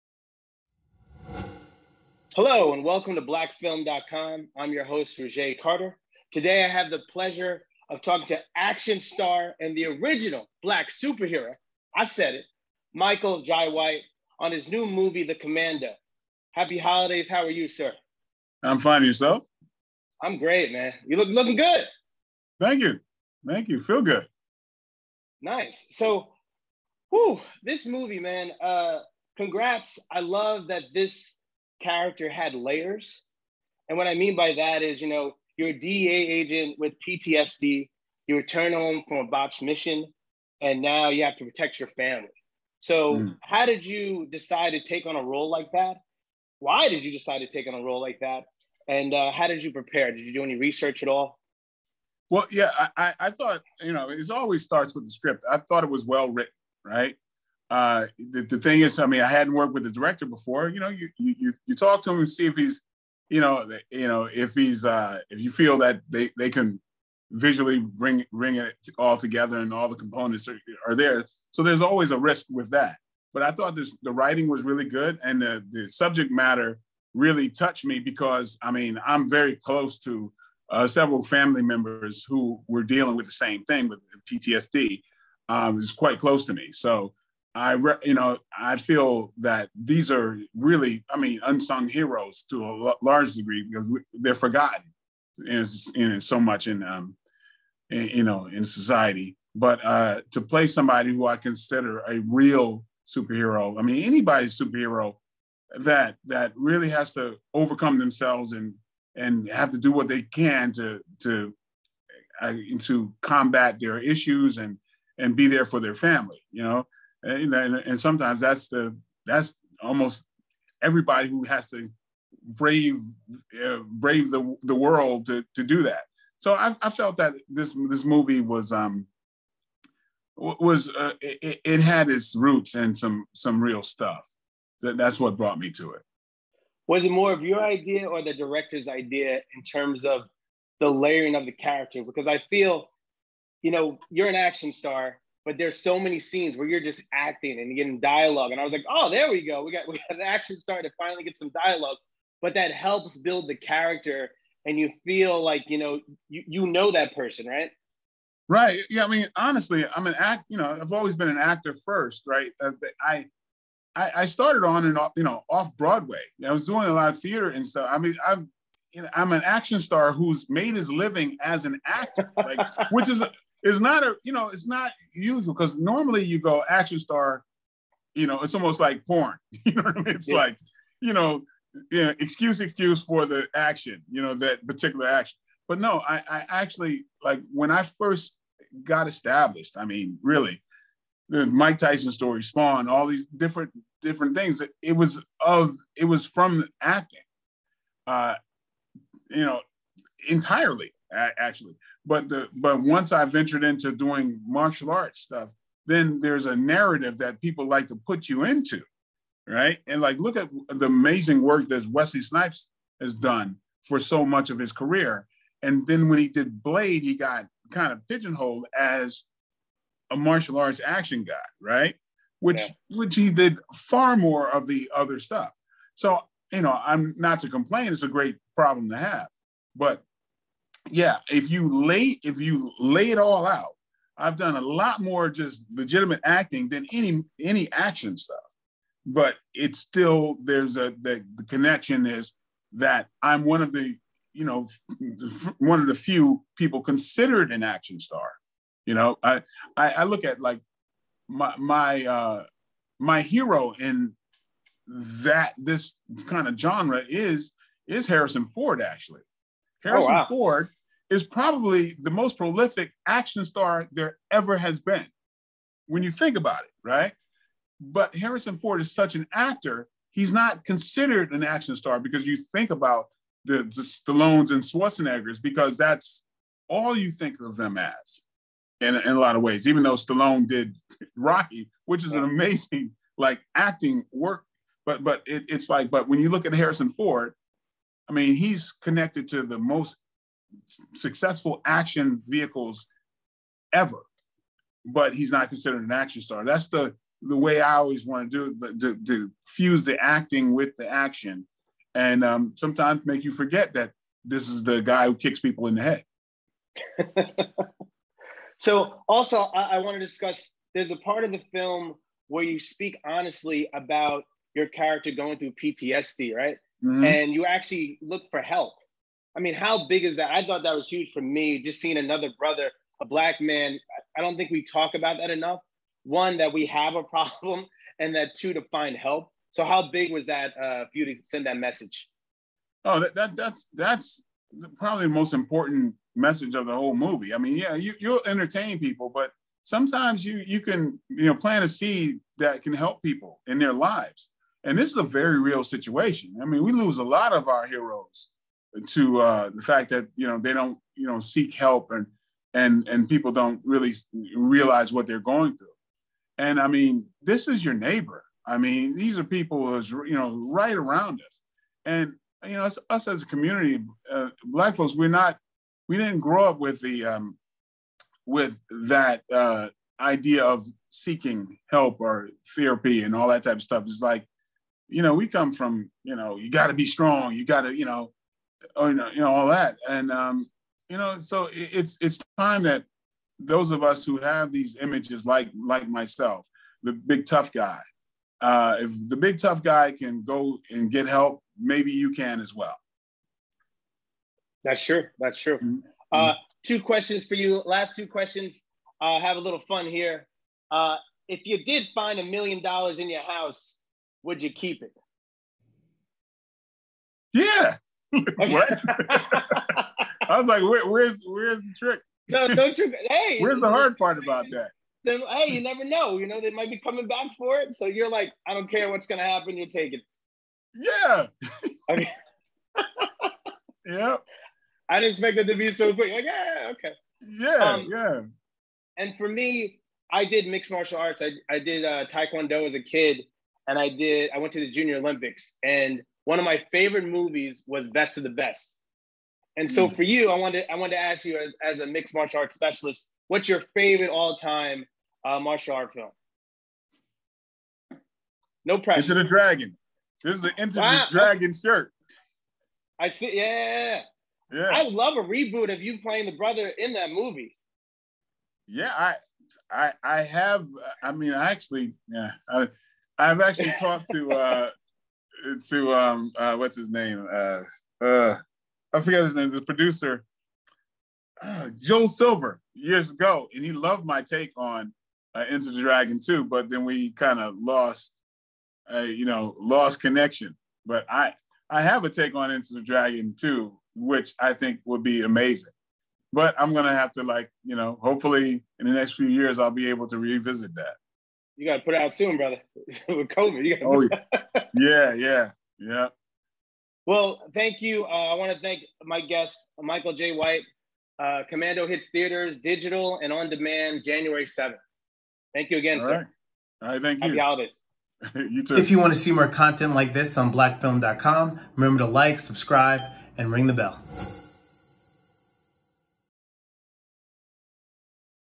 Michael Jai White talks 'The Commando'
blackfilm.com correspondent Ro Carter talks to action film hero & superstar Michael Jai White about his new film 'The Commando' opposite Mickey Rourke! The pair get into Michael Jai White's beginnings & some of his favorite films in the genre.

Synopsis:  In this edge-of-your-seat crime thriller starring Michael Jai White and Mickey Rourke, an elite DEA agent (White) returns home after a failed mission when his family makes an unexpected discovery in their house – a stash of money worth $3 million. They soon face the danger and threat of a newly released criminal (Rourke) and his crew who will do whatever it takes to retrieve the money – including kidnap the agent's daughters. Stakes are high and lives are at risk in this head-to-head battle as the agent stops at nothing to protect his family against the money-hungry criminals.